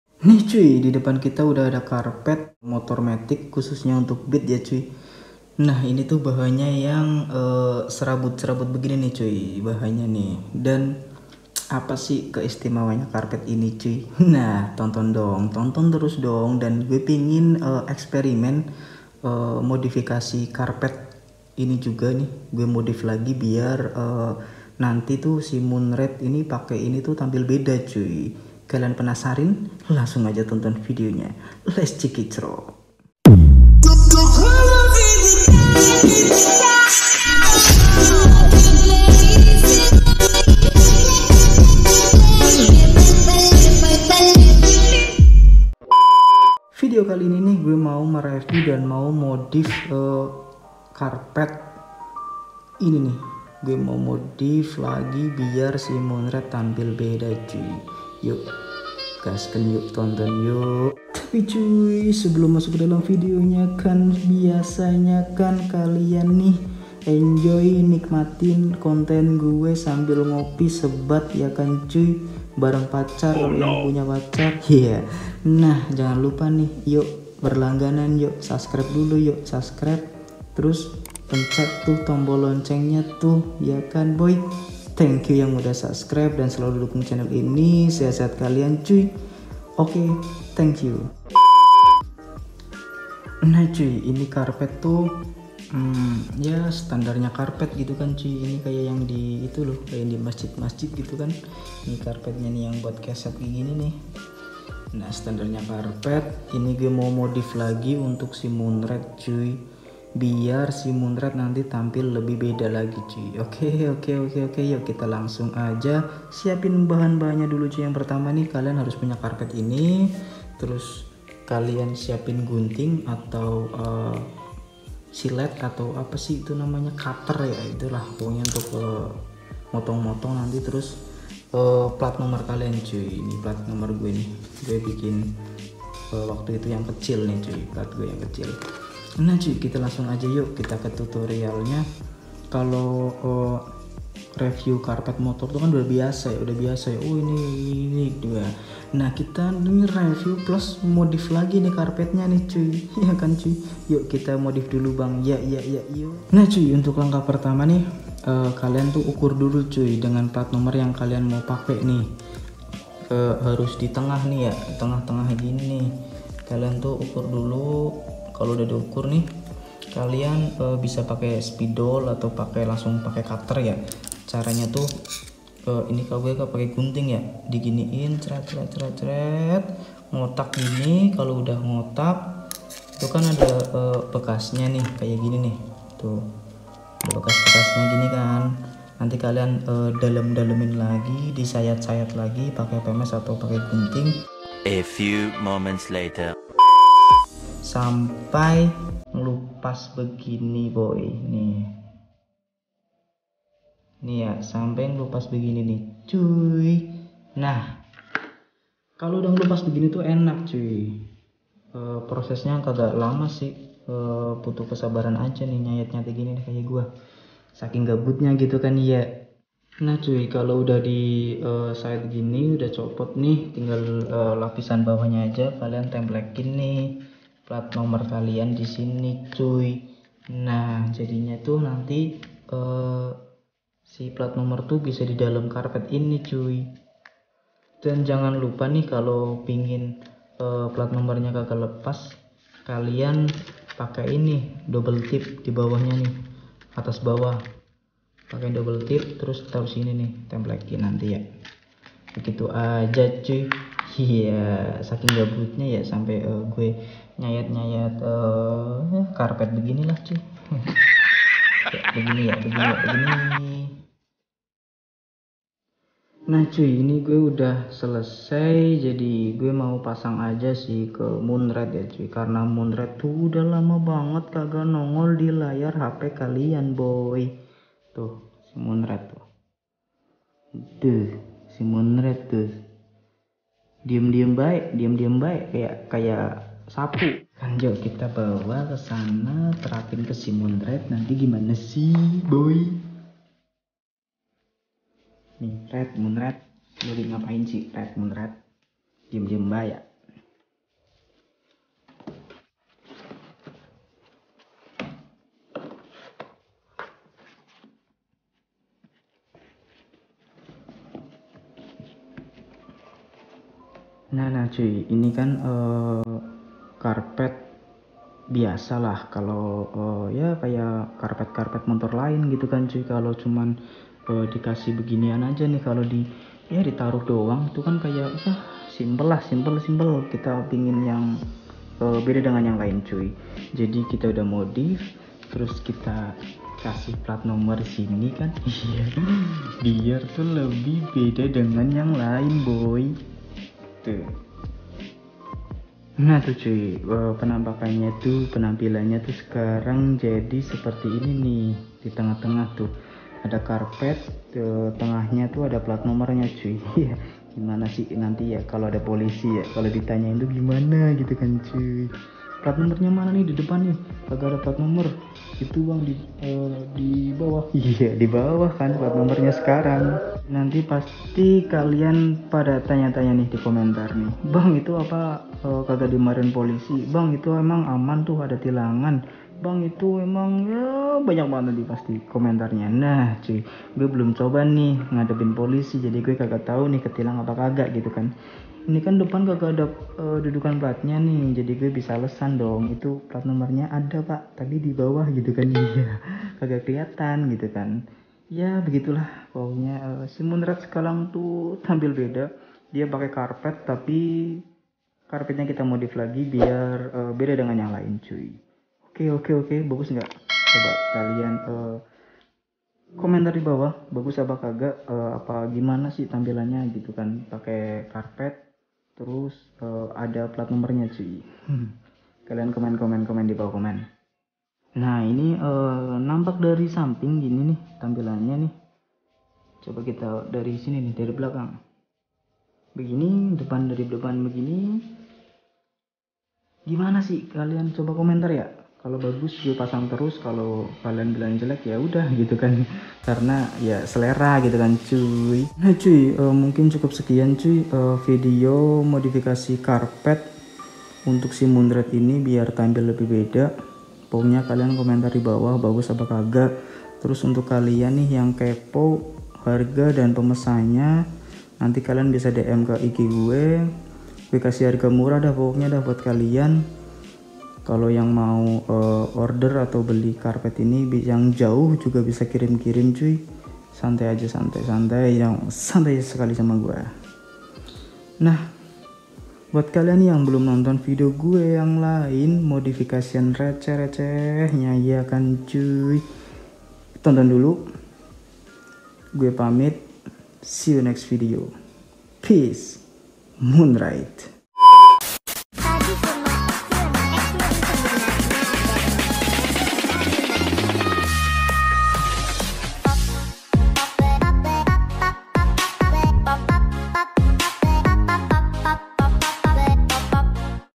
REVIEW + TUTORIAL MODIF KARPET MOTOR MATIC,BIKIN MOTOR ELEGAN DAN KEREN 🔥
Hi boy kembali lagi dengan Boy Xenpai yakan cuyy, Barang baru lagi nih boyy

modifikasi honda beat
modifikasi beat 2020
modif beat new cbs iss
modif beat 110cc
Karpet motor
Karpet motor matic
Modifikasi krpet
Modif karpet motor
Beat 2020 modifikasi